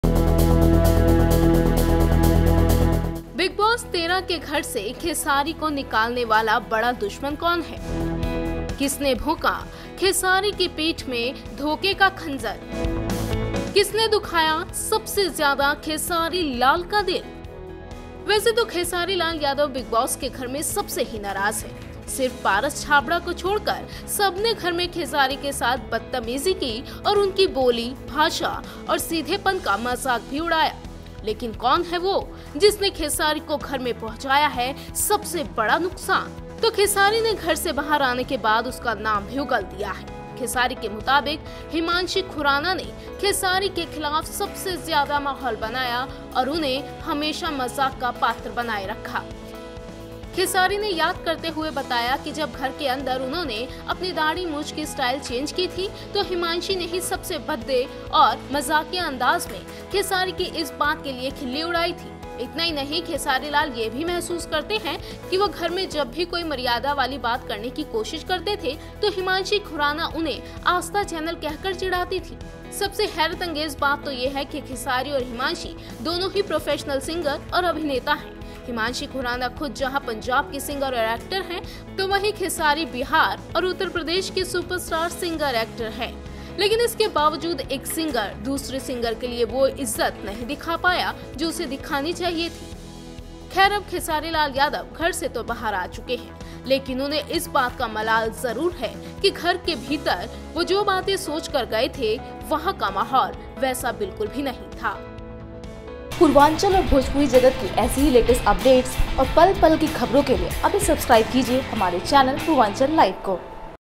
खेसारी ने किया घर के सबसे बड़े “नकली” इंसान का खुलासा !
देखिए बिग बॉस के घर से निकलने के बाद खेसारी लाल ने घर के किस सदस्य को अपनी जिंदगी का सबसे बड़ा विलेन बताया है ?